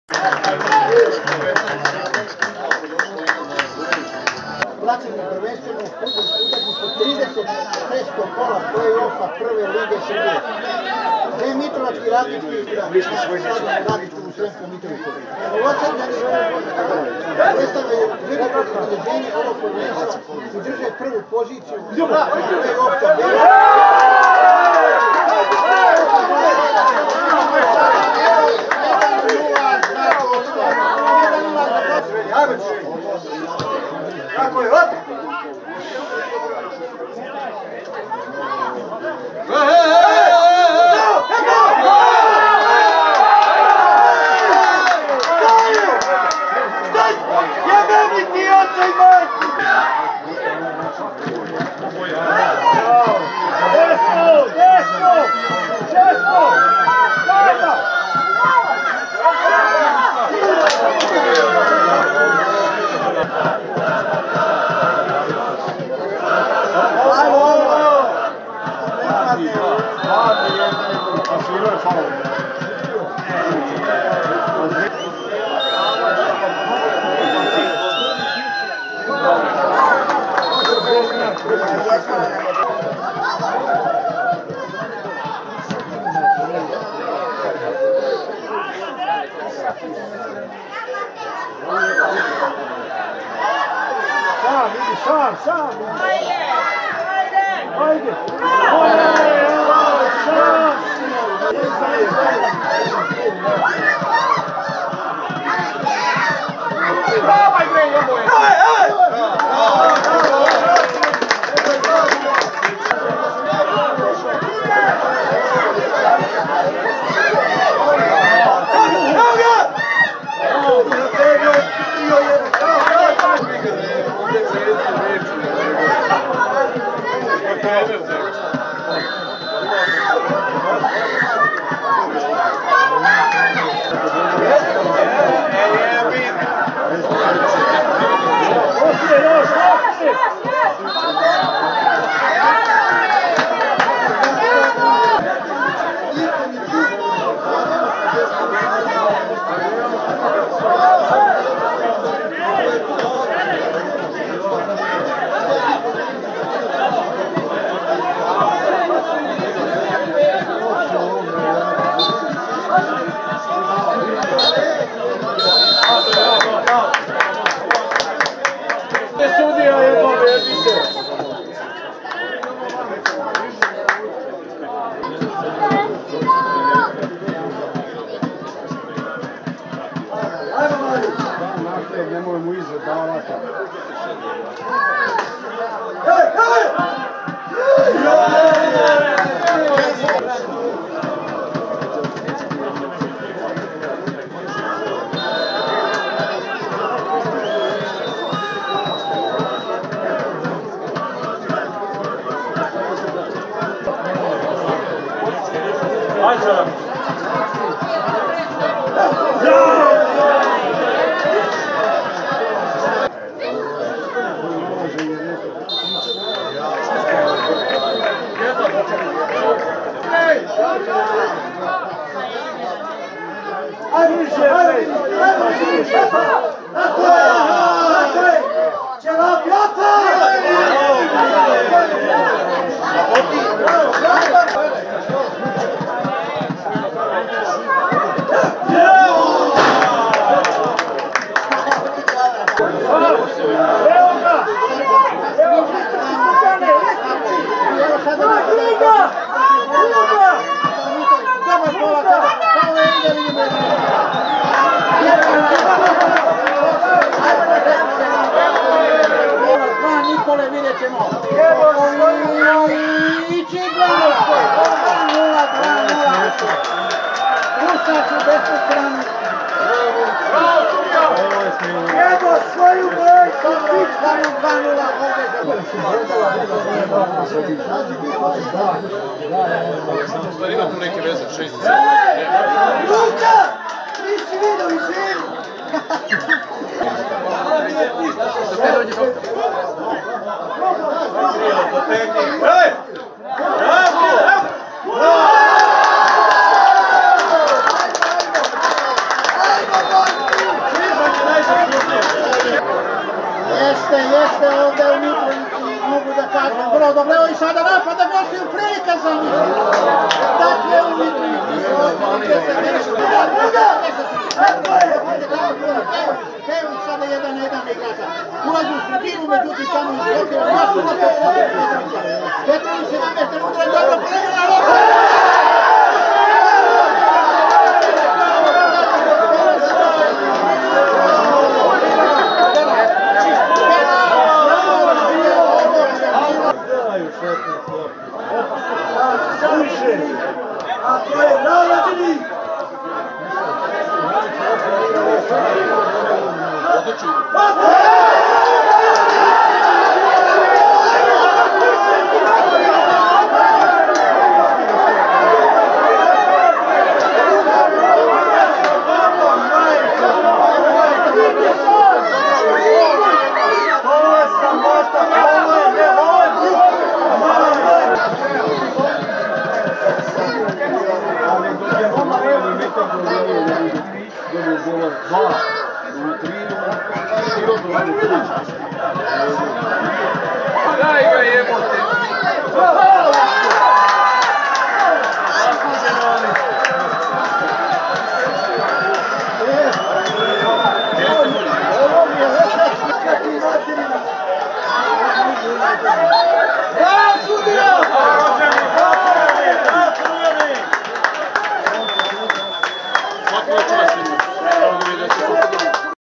Prva liga Srbije 2021/22: 36.Kolo - RADNIČKI SM – MLADOST NS 1:3 (0:1)
Strelci: Ognjen Dimitrić 72' - Milan Vidakov 4', Nikola Popović 75', Miloš Savanović 90'